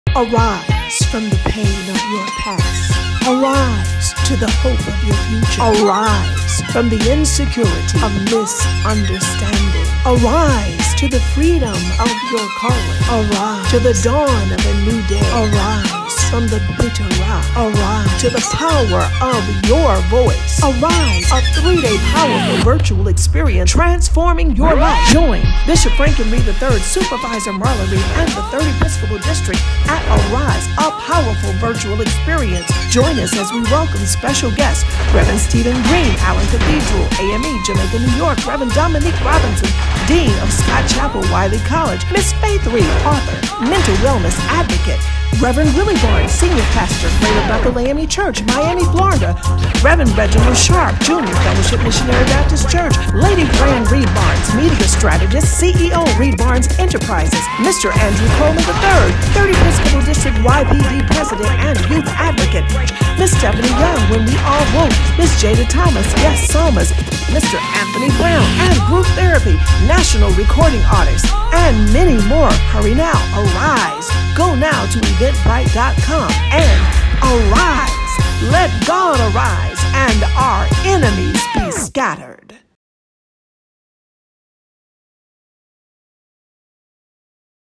ARISE Conference video
Register NOW for ARISE hosted by The 3rd Episcopal District of the AME Church, Bishop Frank Reid, III and Episcopal Supervisor Marlaa' Reid, hosts.